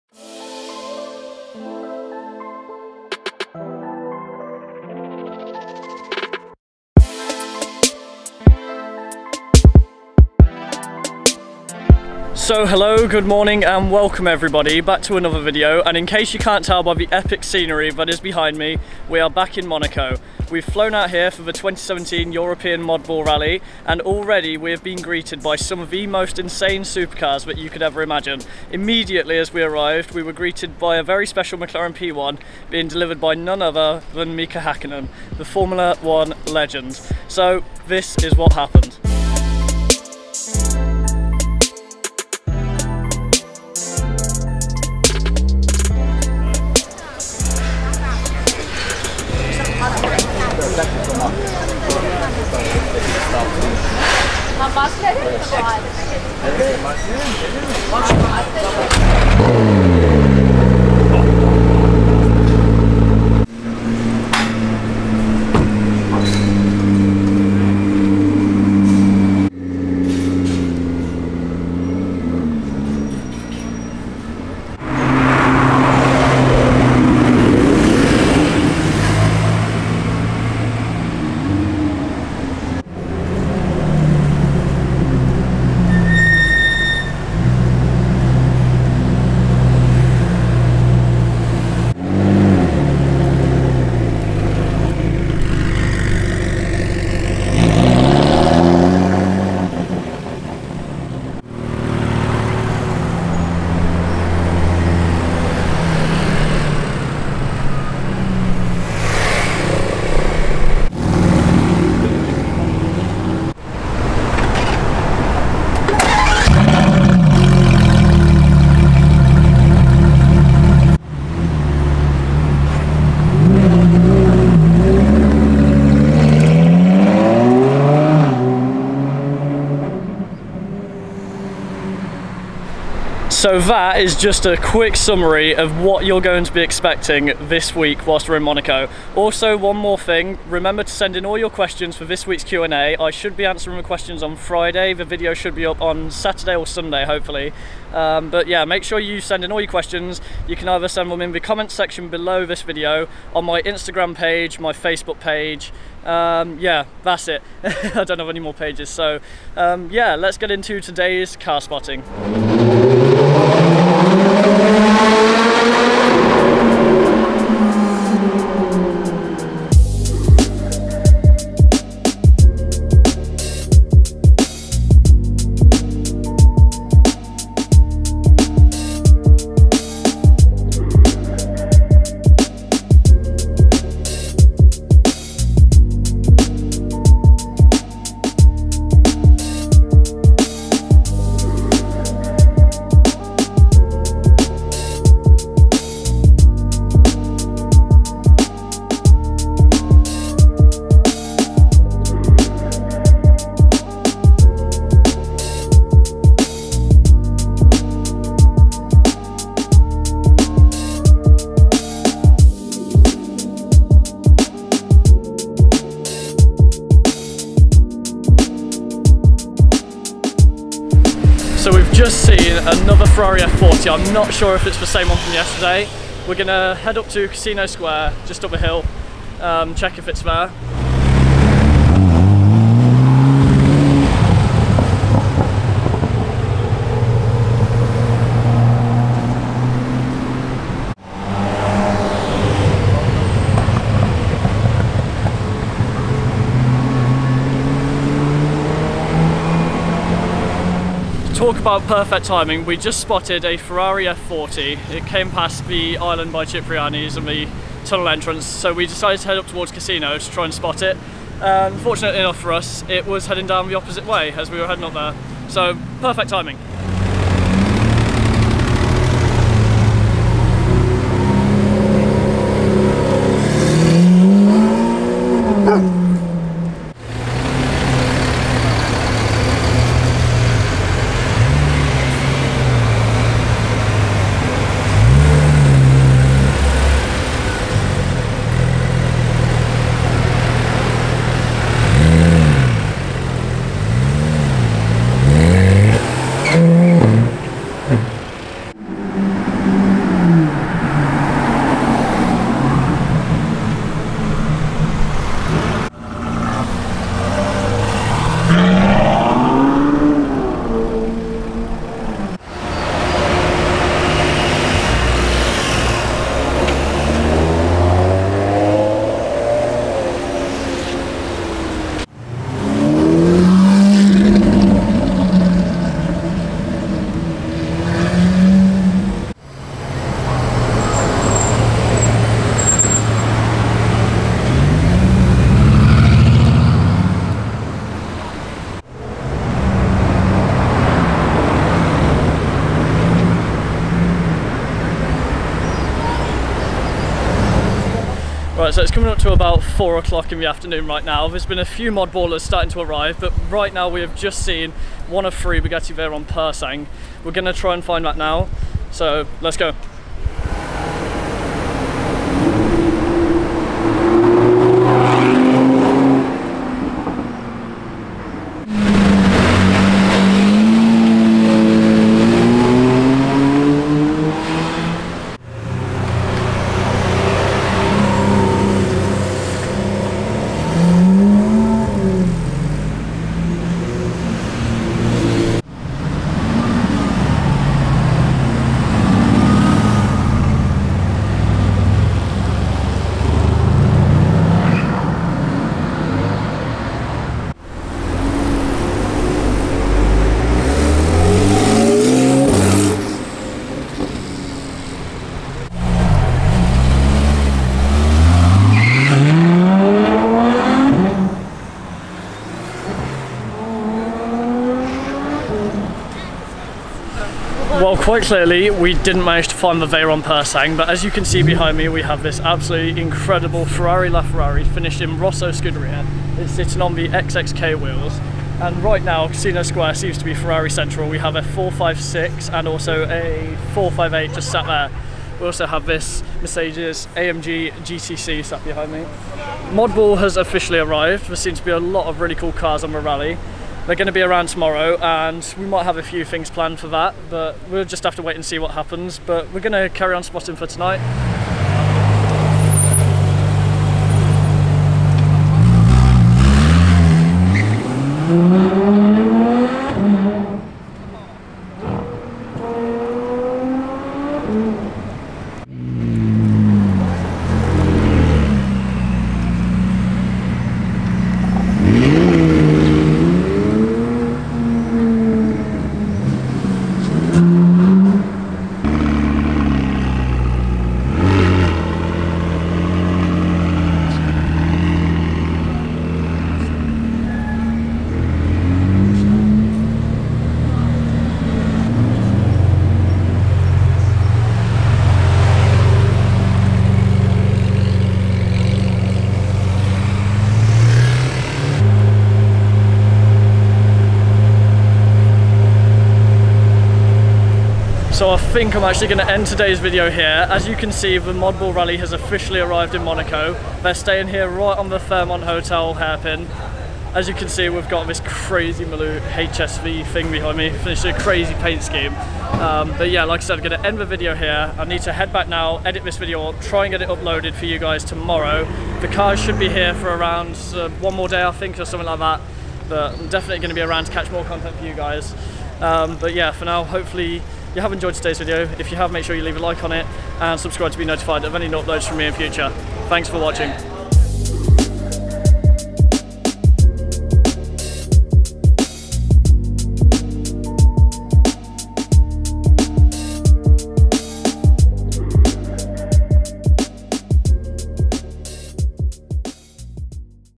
Mika Hakkinen Driving His McLaren P1 in Monaco! [Monaco Supercar Insanity #5]
Welcome back to Monaco Supercar Insanity!

We are finally back in Monaco, this time for the 2017 European Modball Rally. Immediately we are greeted by a bunch of insane supercars including Mclaren P1 VP3 being driven by F1 Legend Mika Hakkinen!

Comment your thoughts and opinions.